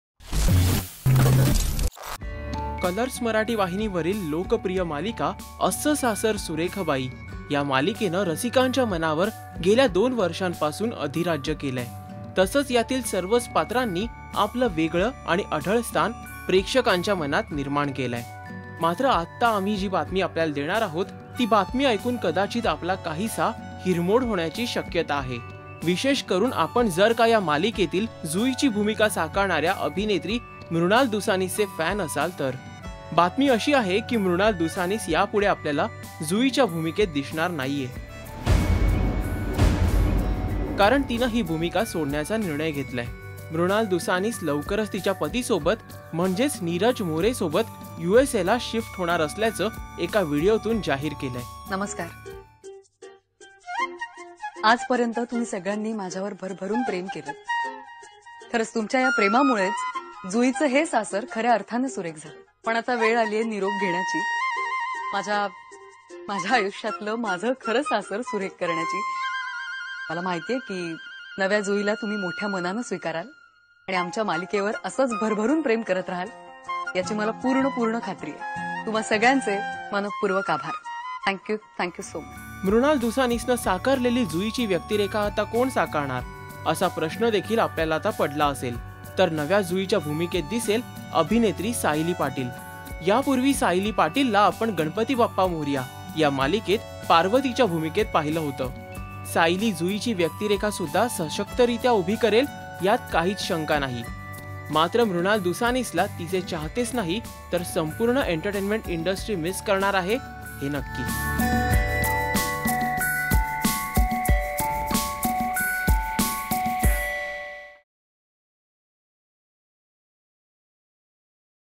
Why Mrunal Dusanis Left Asa Sasar Surekh Bai Serial ? | Colors Marathi
Lead actress Mrunal Dusanis from popular marathi serial Asa Sasar Surekh Bai Serial has decided to quit her role as Jui. What is major reason for taking such a big step ? Check out video to know more only on Marathi Showbuz.